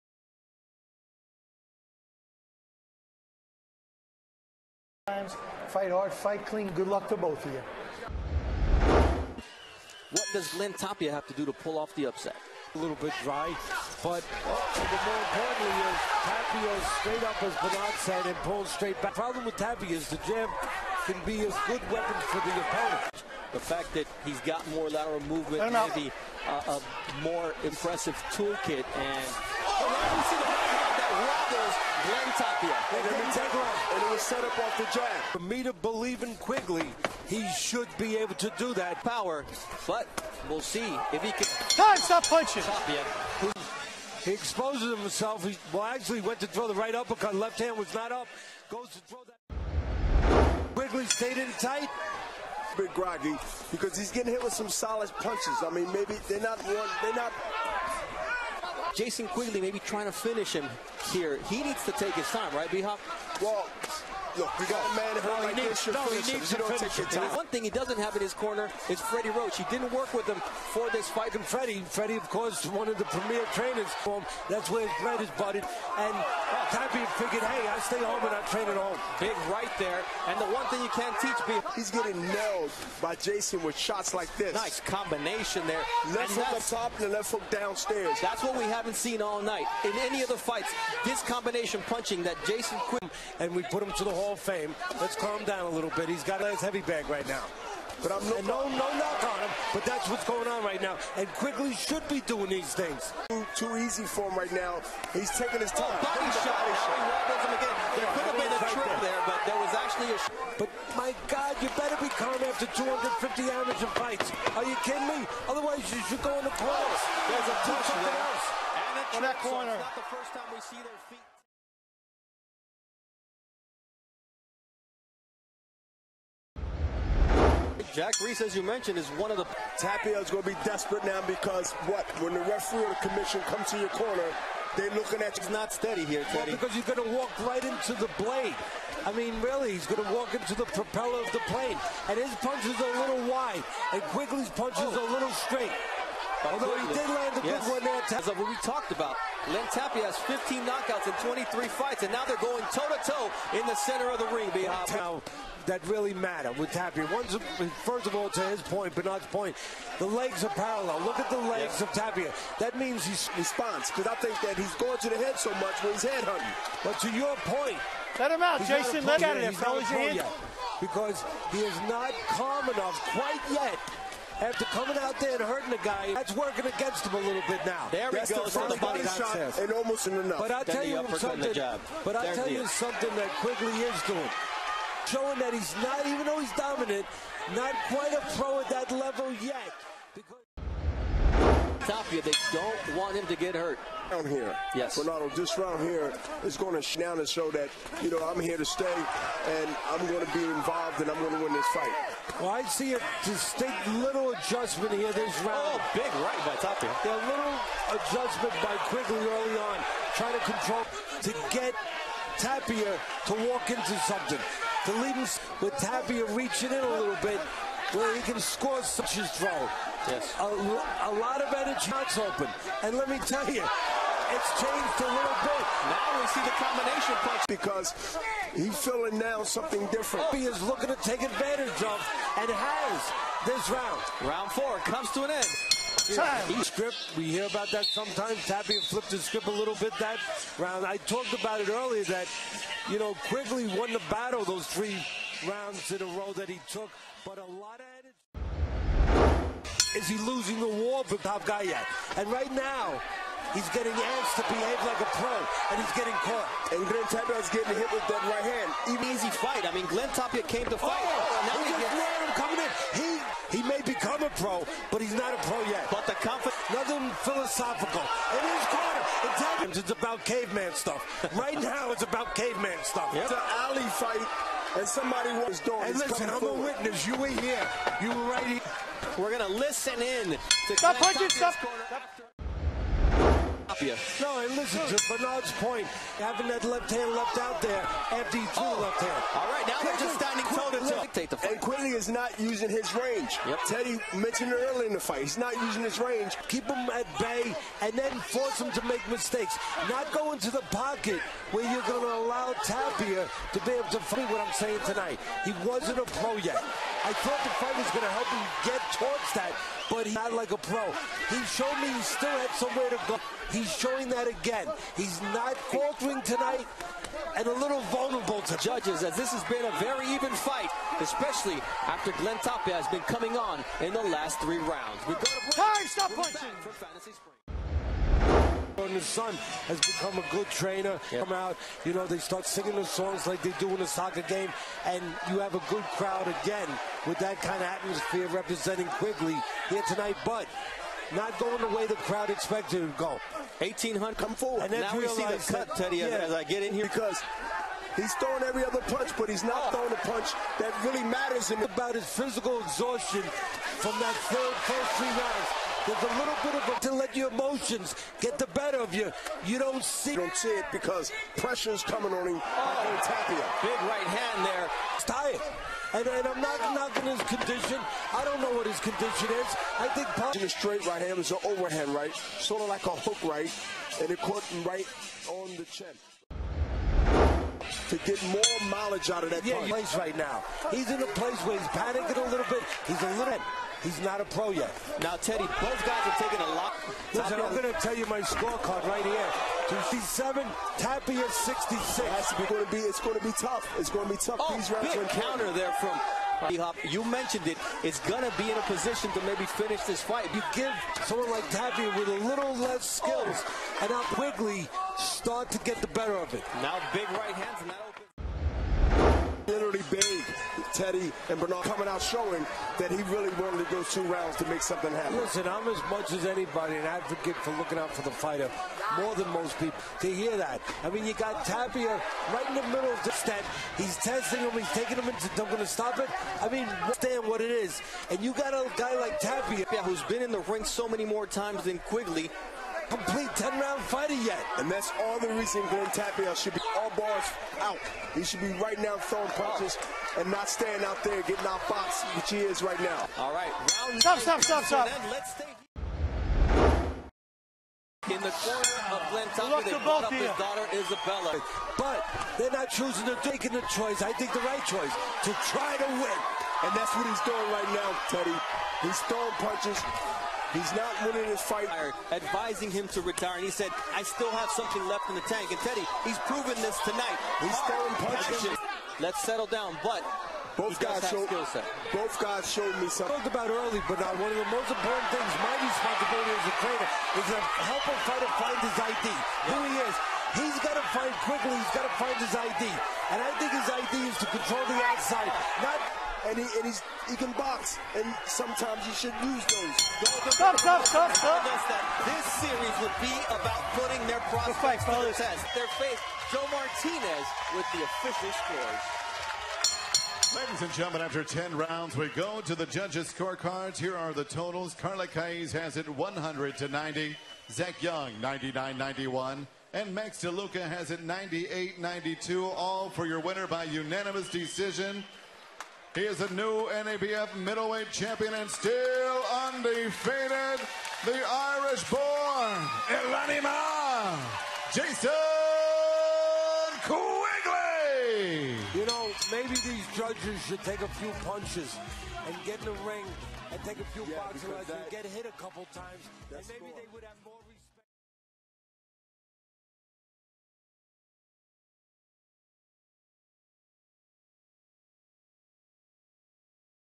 Jason Quigley vs Glen Tapia: Highlights
Highlights of the Middleweight war between Unbeaten Irish prospect Jason Quigley and veteran Glen Tapia of New Jersey.

I OWN NONE OF THE FOOTAGE!!!